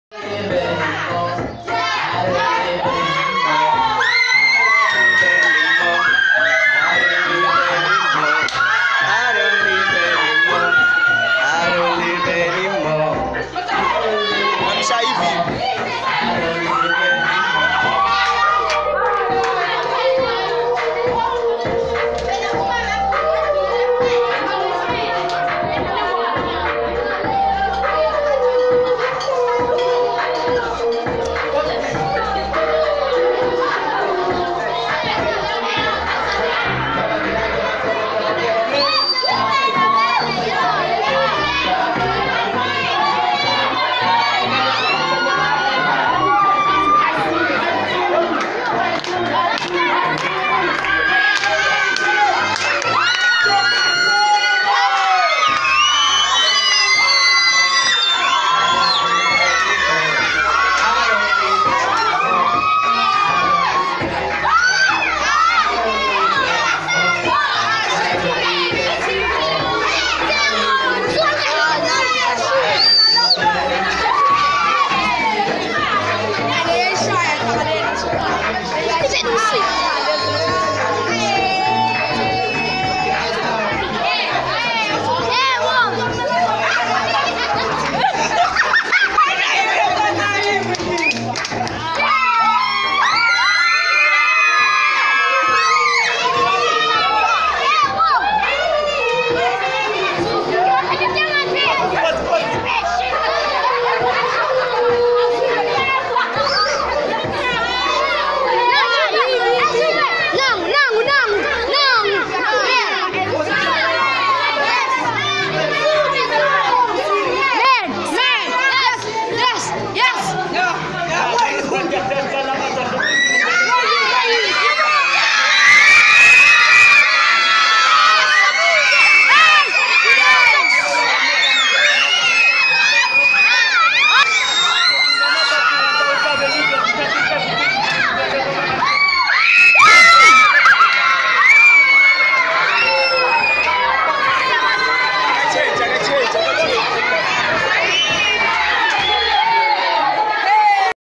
Job maseko primary school ft KDaxoul & T craxy.com (2019/11/23)
Farewell for lovely Grade 7's